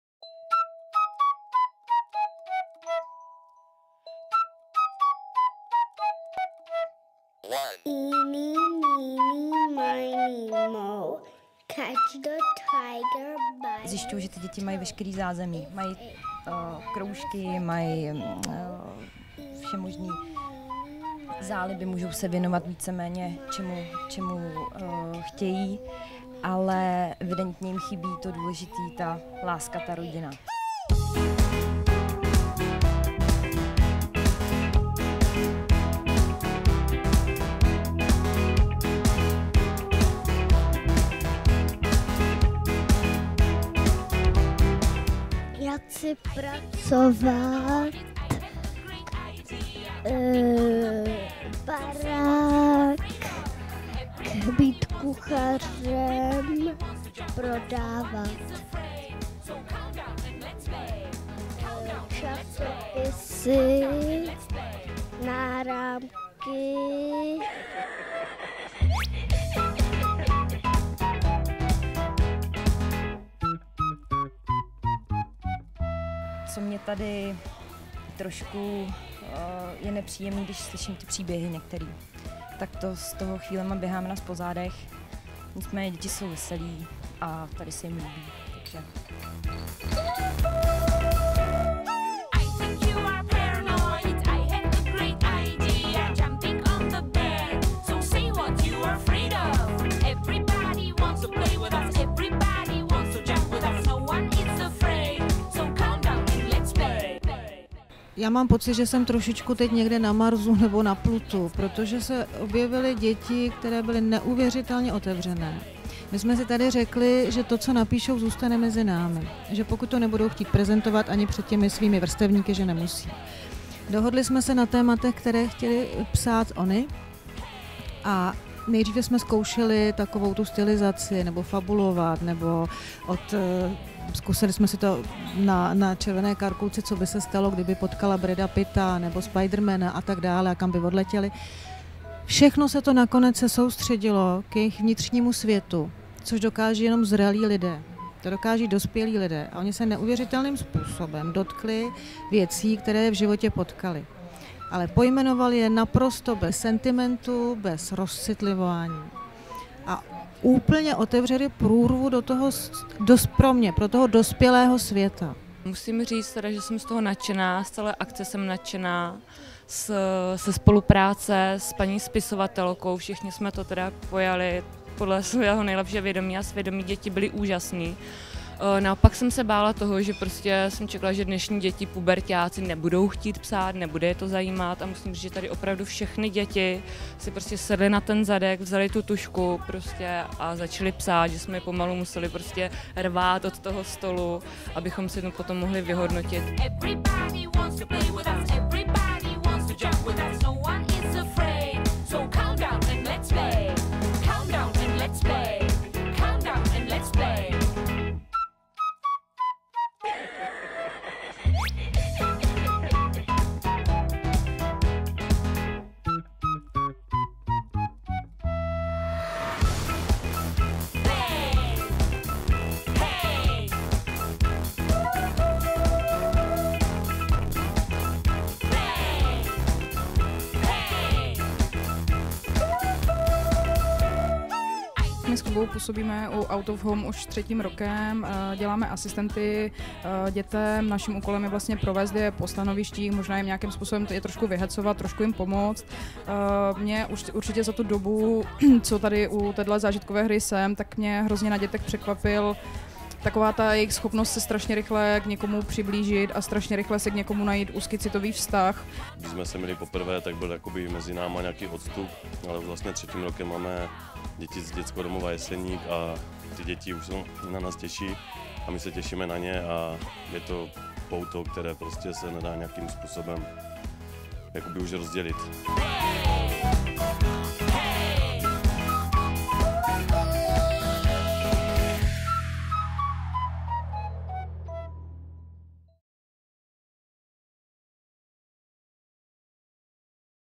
Out Of Home 2012 reportáž
Na festivalu OUT of HOME se podílí celá řada dobrovolníků. Někteří z nich se starají o týmy z dětských domovů, jiní pomáhají na stanovištích zážitkové hry jako instruktoři, nebo působí jako lektoři. OUT of HOME je zaměřený na děti z dětských domovů a hlavním cílem je, podpořit jejich integraci do běžného života ve chvíli, kdy opuští dětský domov.